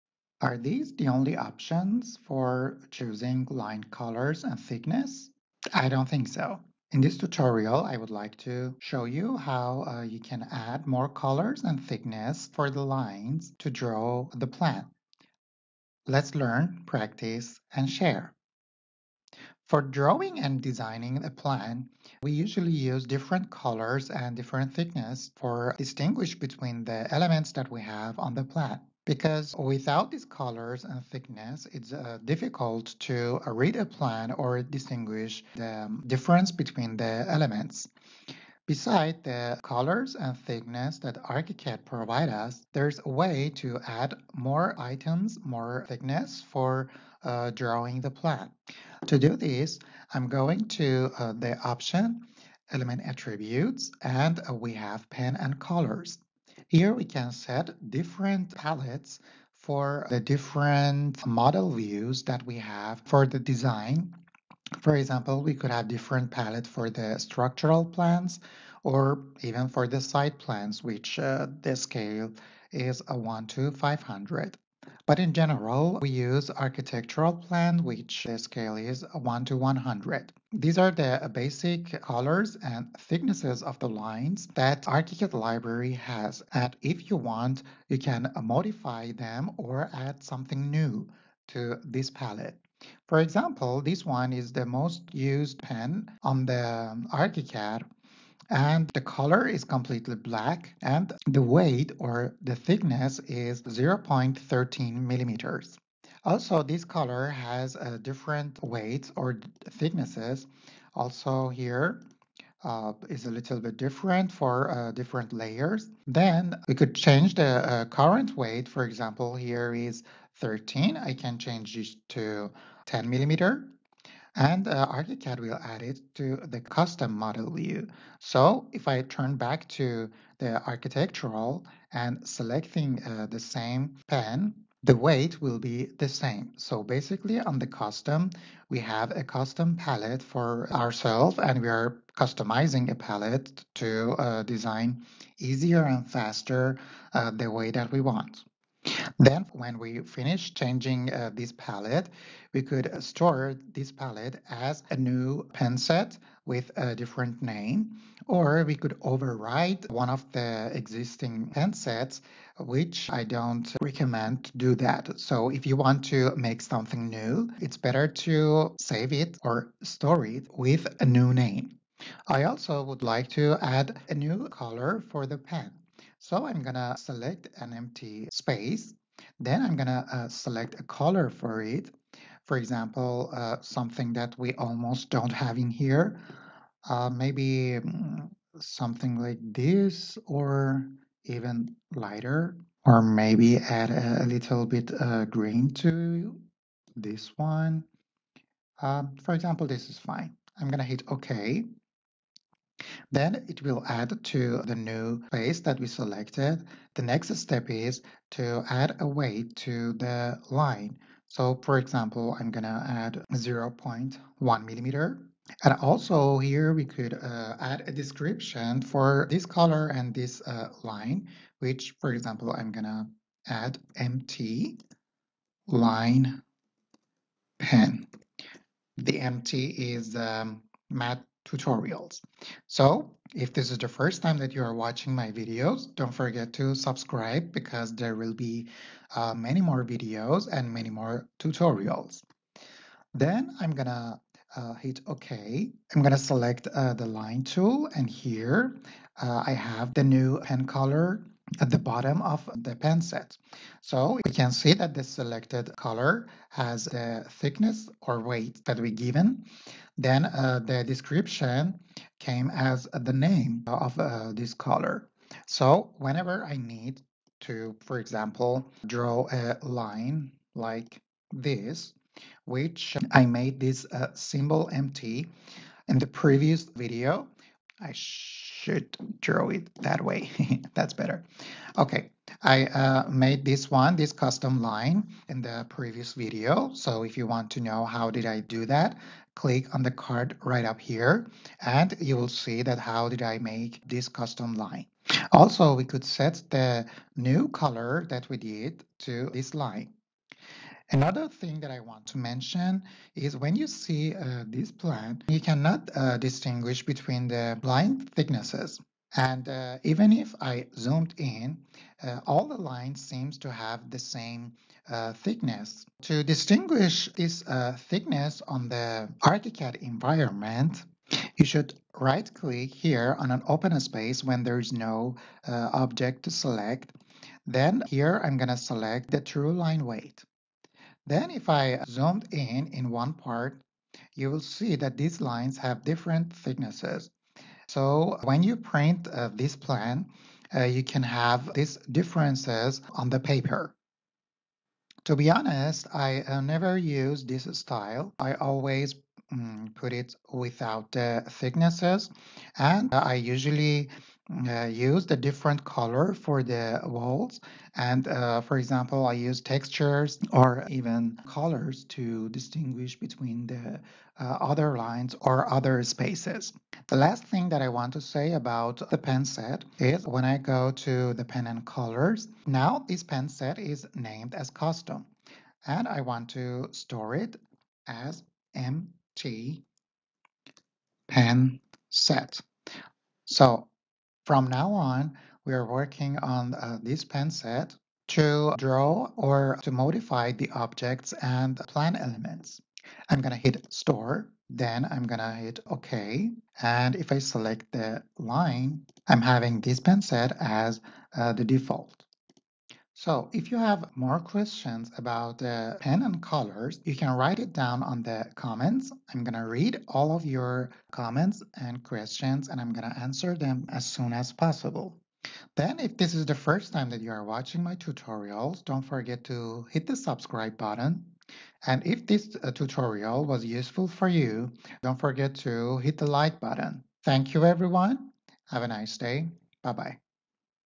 Archicad - How to make a new pen and colors [line weight and pen set]
In this video, I explain how you can make new line weights and colors. also you can modify the existing pens and store them as a new pen set.

0:00 Intro

🔔 Make sure you have the bell turned on, so you will definitely not miss any of my videos